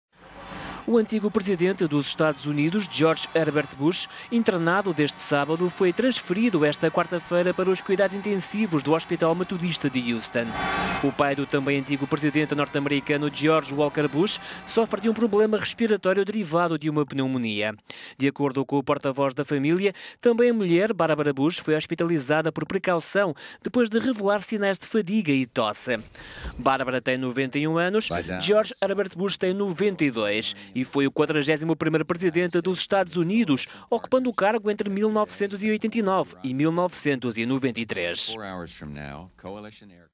41.° Presidente dos Estados Unidos está nos cuidados intensivos
O ex-Presidente norte-americano George Herbert Walker Bush, hospitalizado desde sábado, foi transferido esta quarta-feira para a unidade de cuidados intensivos do Hospital Metodista de Houston devido a um problema respiratório derivado de pneumonia.

De acordo com o porta-voz da família, Jim McGrath, a mulher do antigo chefe de Estado, Barbara Bush, de 91 anos, foi também hospitalizada por precaução, depois de revelar sinais de fadiga e tosse.

  George H.W. Bush is in stable condition at a Hous…